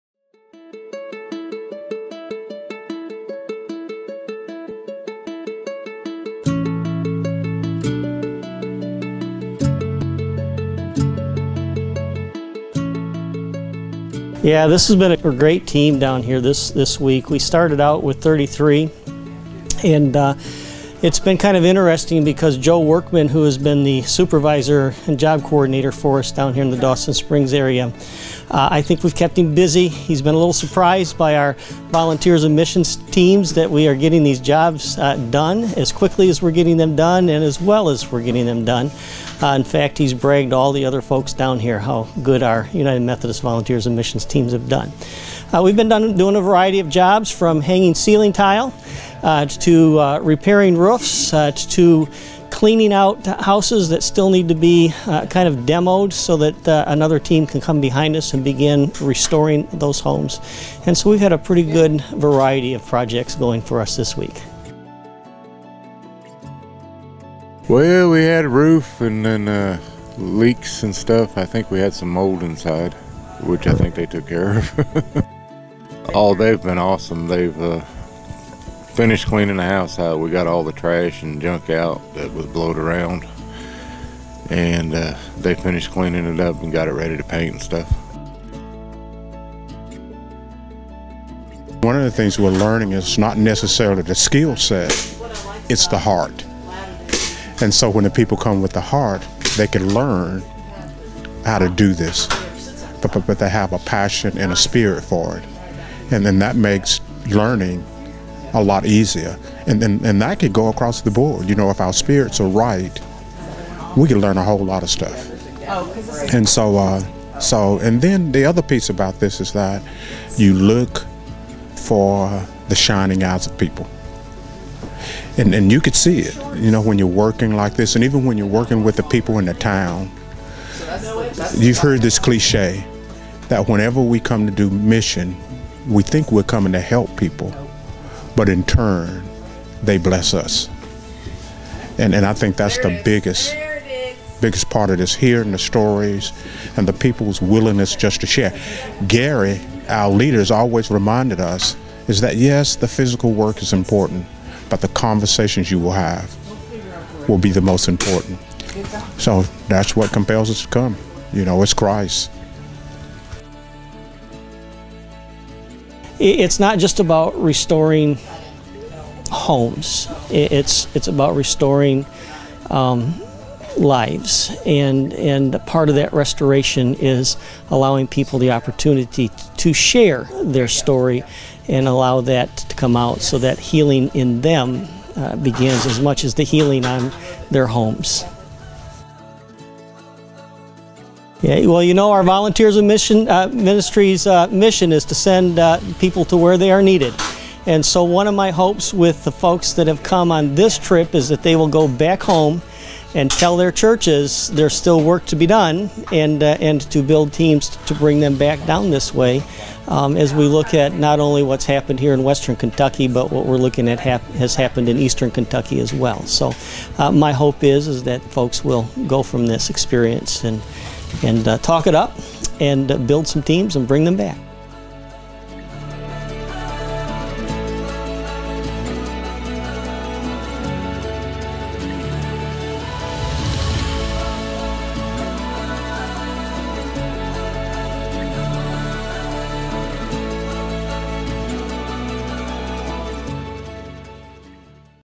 October 2022 Western KY Mission Trip
Bishop Leonard Fairley and the Appointive Cabinet of the KY Annual Conference participate in a Volunteers in Mission trip to the areas damaged by the December 2021 tornadoes that ravaged the western part of our conference.